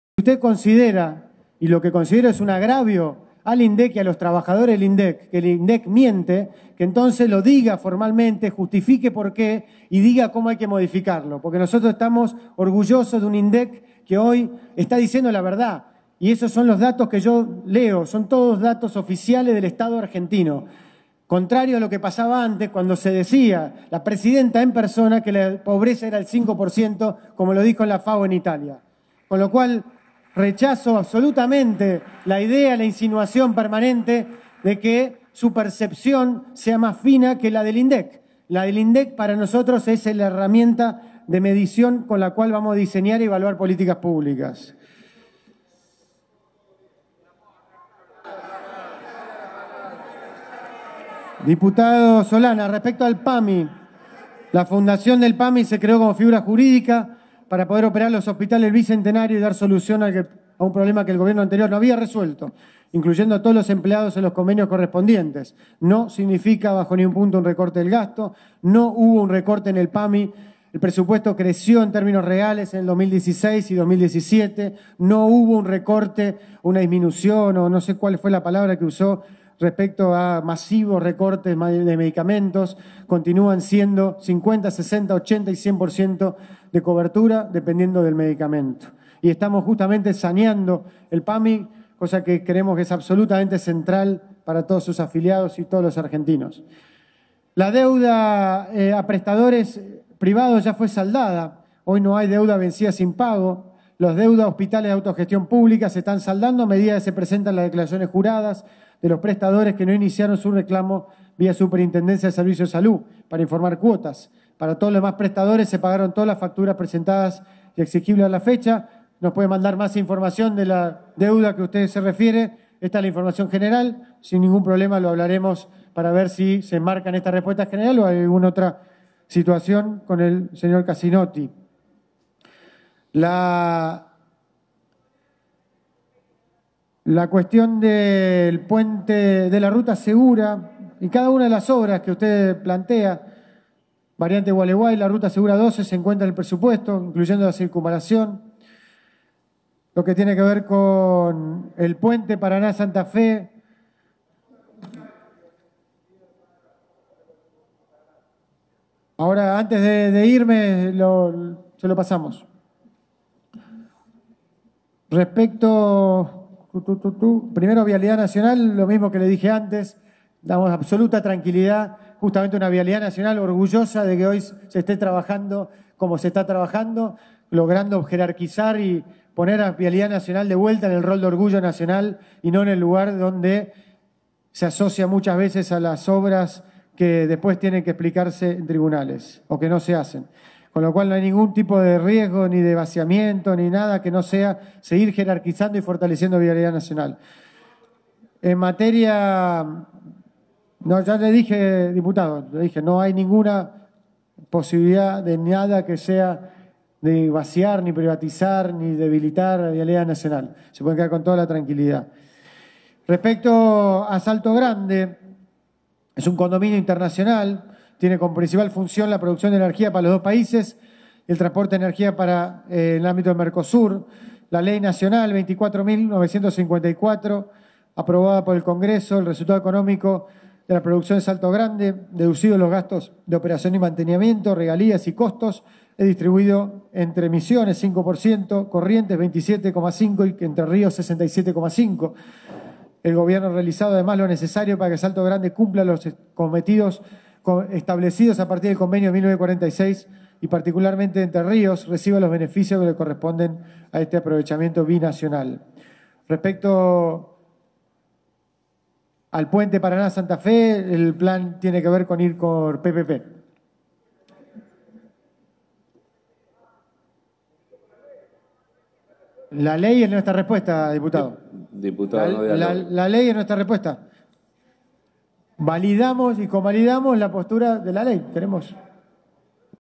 Peña habló sobre Salto Grande en Diputados
Solanas le preguntó sobre la provincialización.